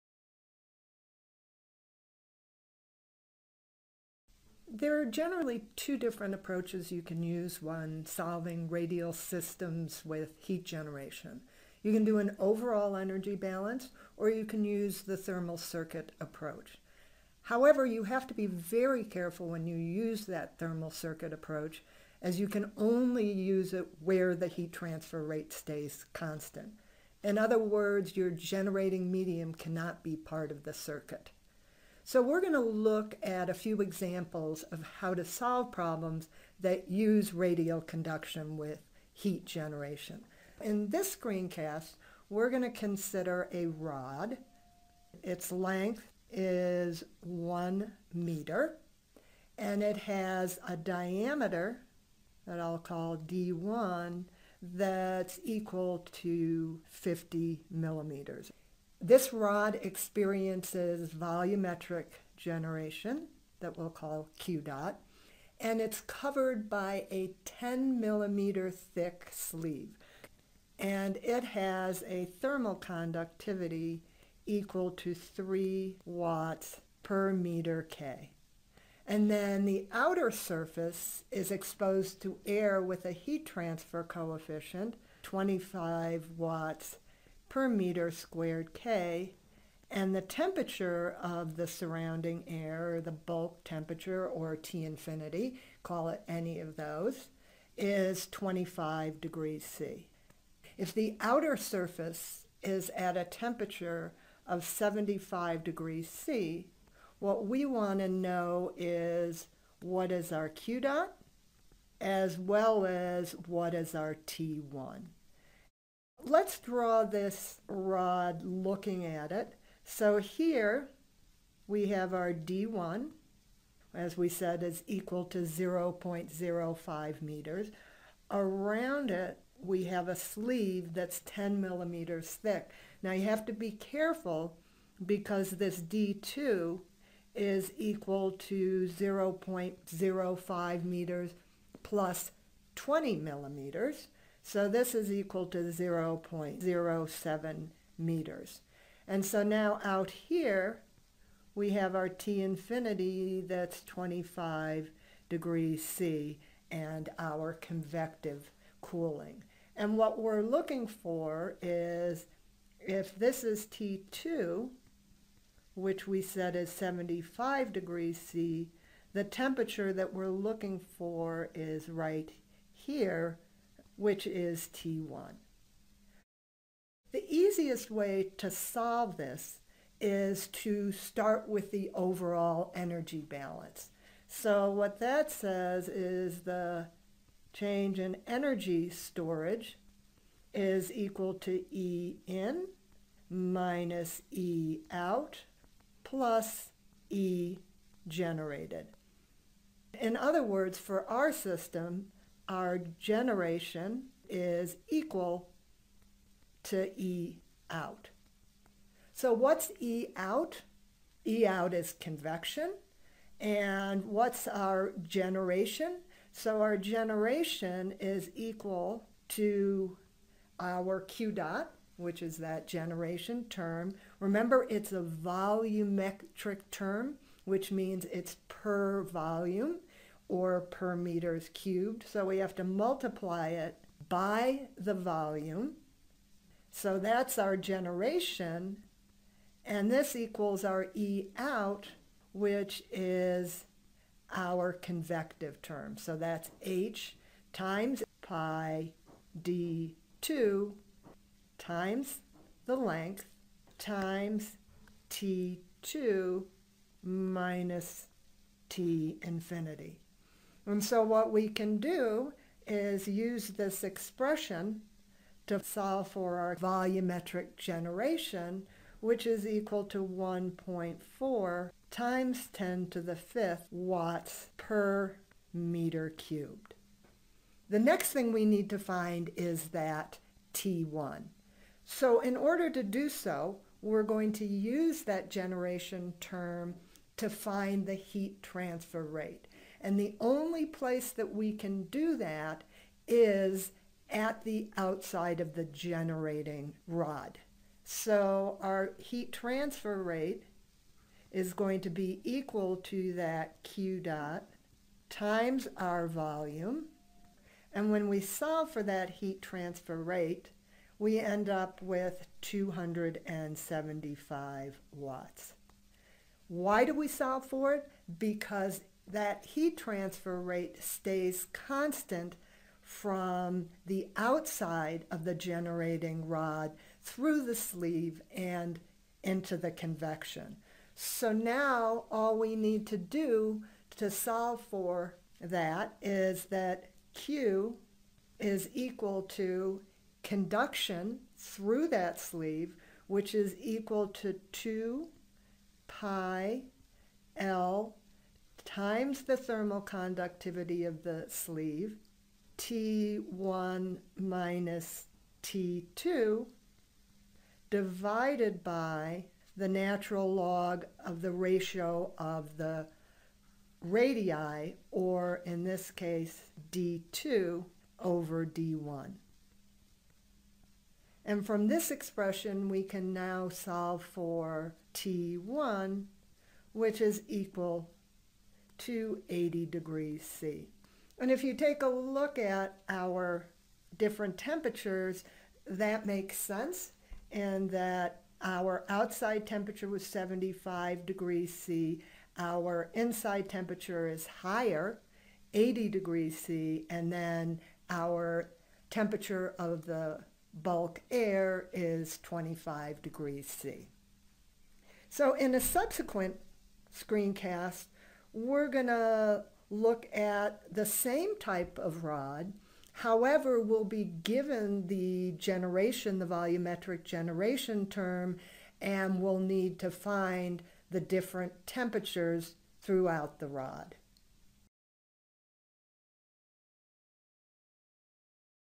Radial Conduction with Generation Part 1
Organized by textbook Calculates the volumetric generation and surface temperature of a rod using an overall energy balance. Made by faculty at the University of Colorado Boulder Department of Chemical and Biological Engineering.